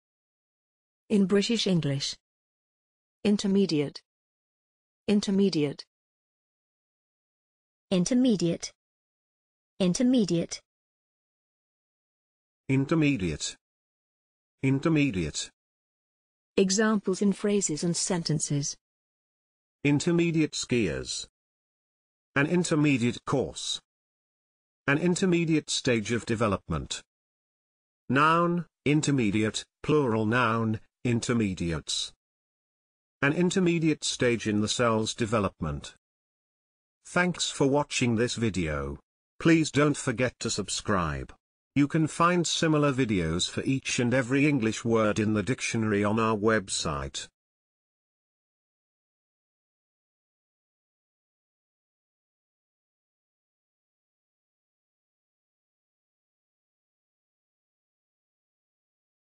intermediate - pronunciation + Examples in sentences and phrases
This is the pronunciation of intermediate in British English + a few examples of its pronunciation in phrases and sentences.